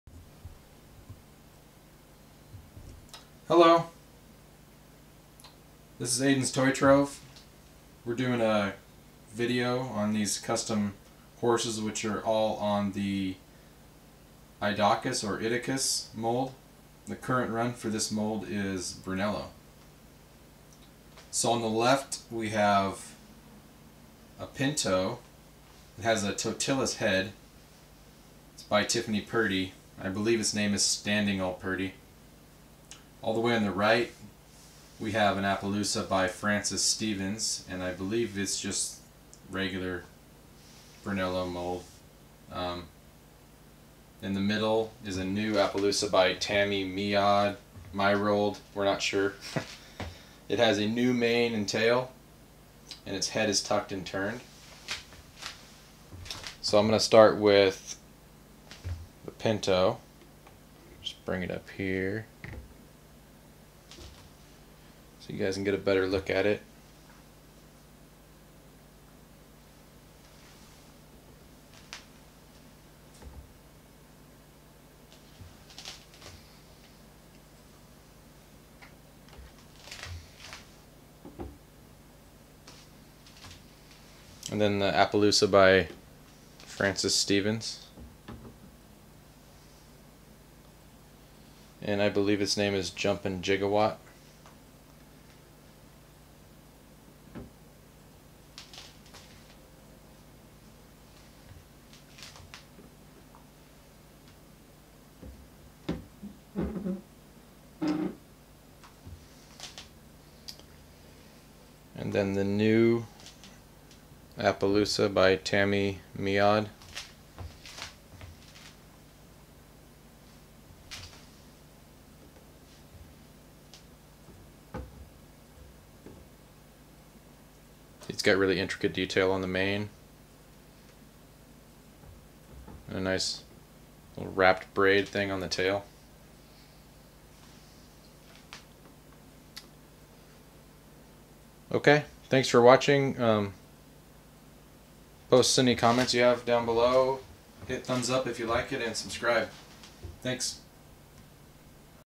Breyer Customs on the same Idocus Mold by 3 different Artists
Here is a side by side comparison of 3 Custom Breyers on the Idocus Mold.  They represent 3 different Artist's visions and are all gorgeous! On the left we have a Pinto that has been customized with a Totilas Head by Tiffany Purdy. On the right we have an Appaloosa by Frances Stevens, and finally in the middle we have a new arrival Appaloosa by Tammy Myrold.  It has beautifully sculpted mane and tail with braids and its head is tucked and turned!